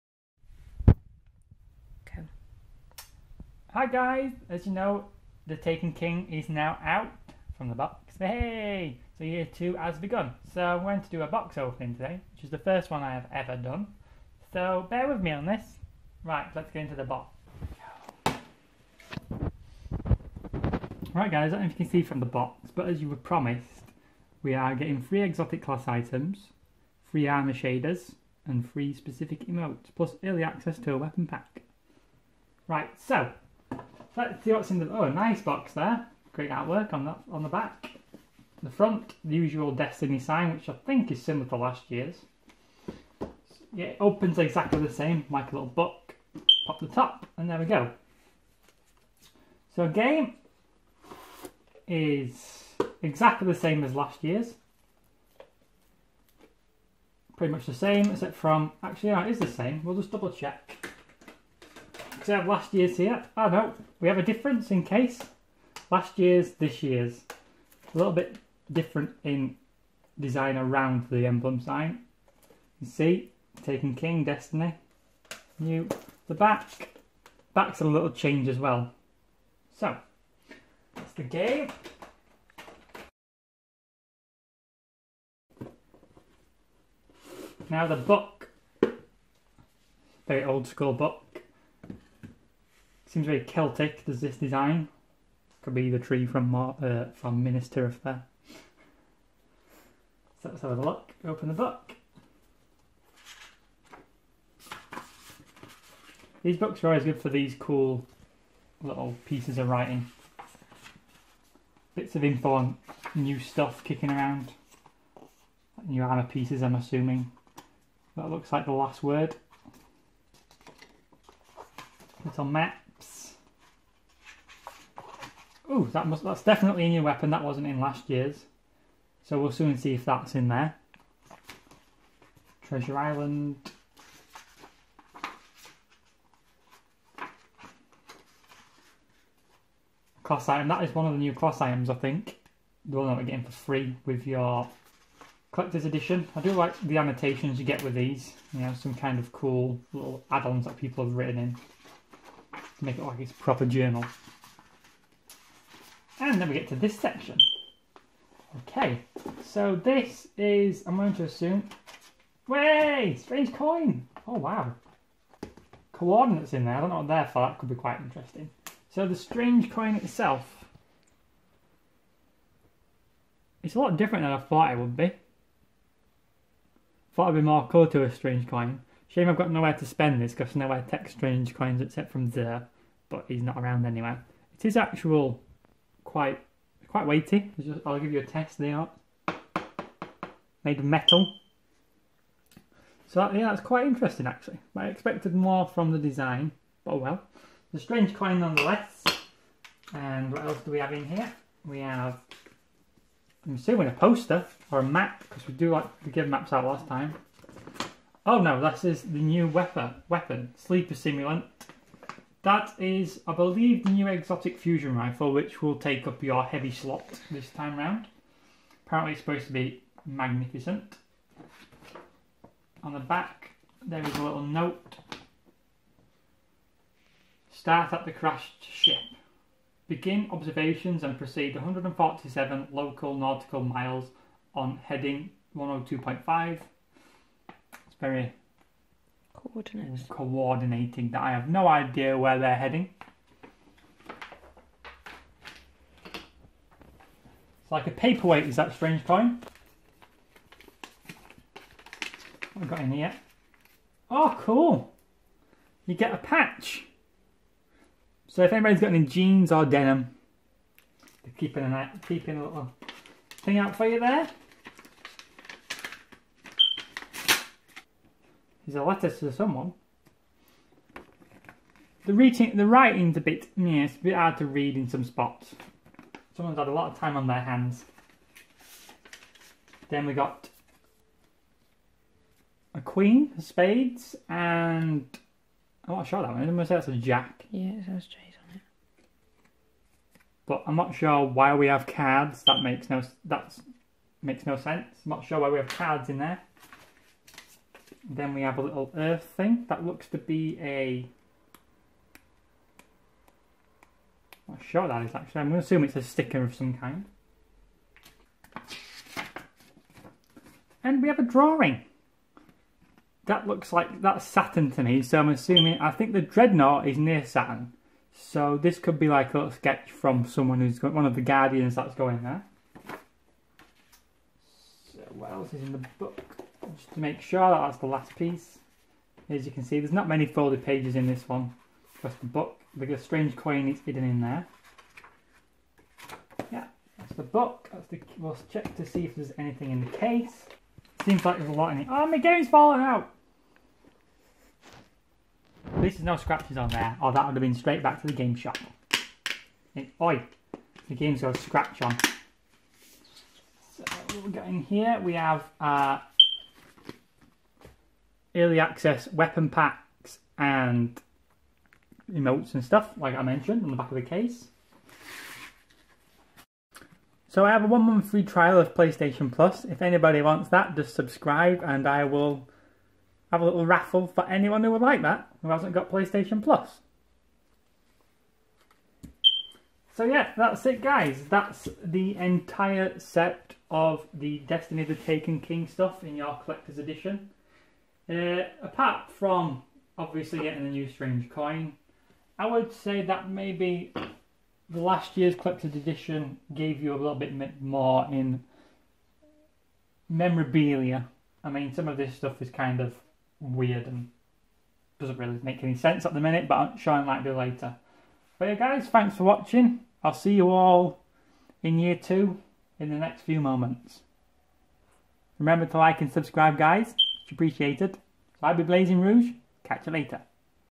The Taken King Collectors Edition Unboxing & 1 Month Free Playstation Plus Giveaway
Sorry for the poor quality but here is the unboxing video of the taken king collectors edition for the taken king, also I will be giving away a 1 months free PlayStation plus trial to 1 of mu lucky subscribers so get subscribing